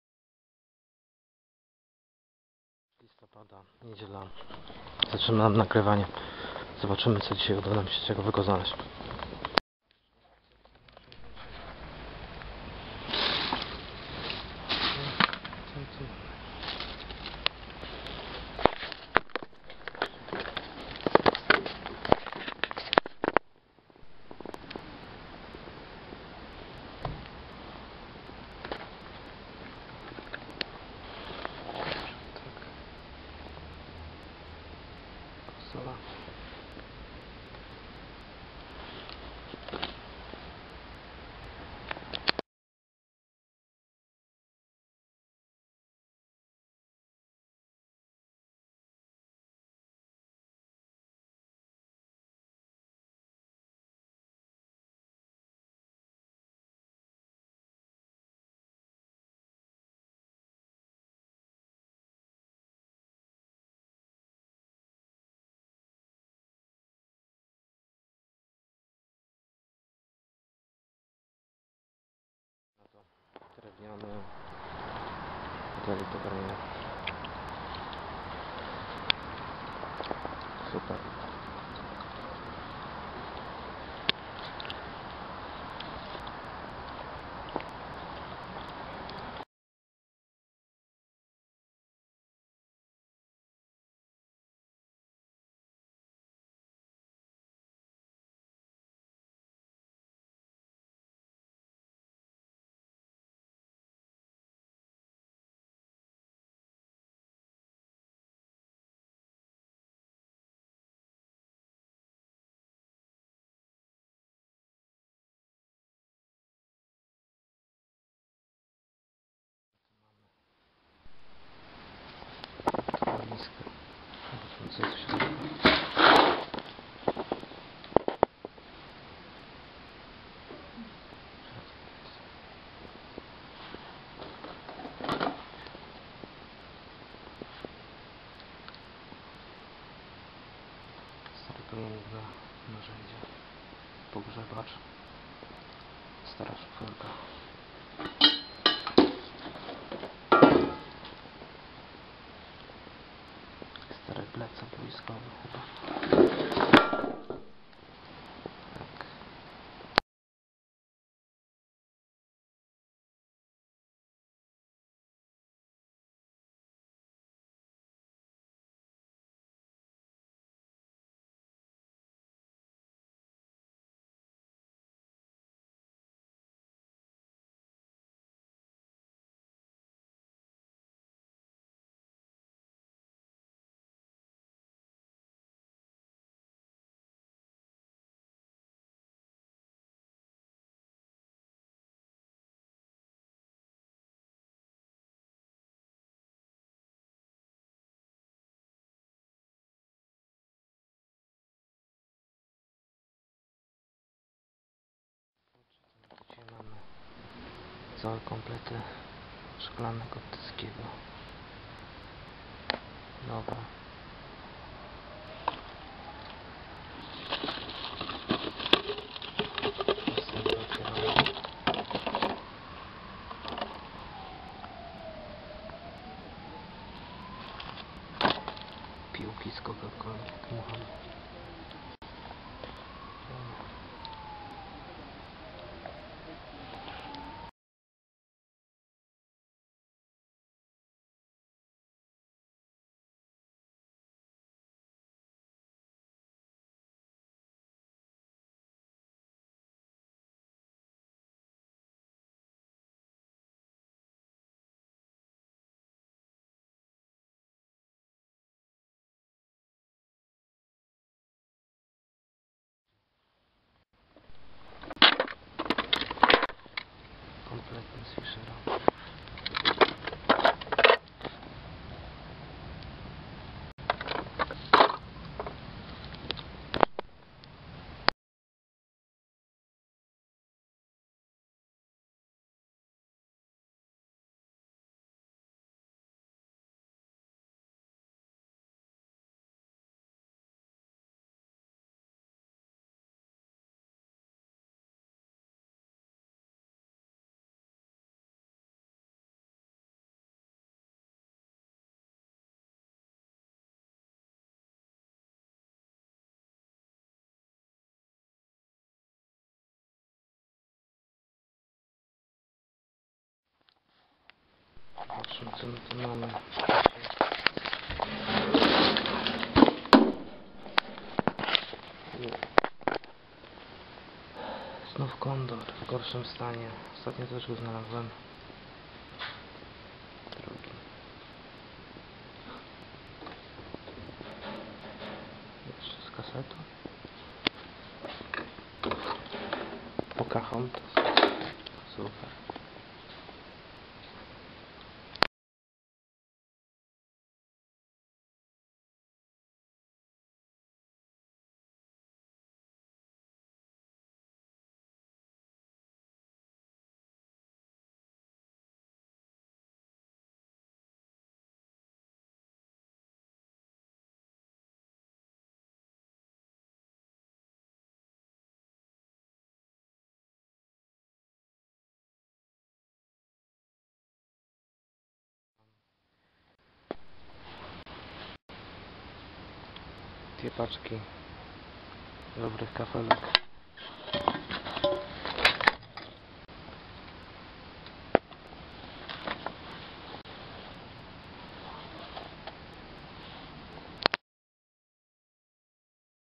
Znalazlem star konsole,sprzet agdPrl,plytki
Znalazlem star konsole,sprzet agdPrl,plytki,śmietnik śmieci hasiok rupiecie wystawki prl Dumpster Diving Poland live 1 listopad 2015 Kraków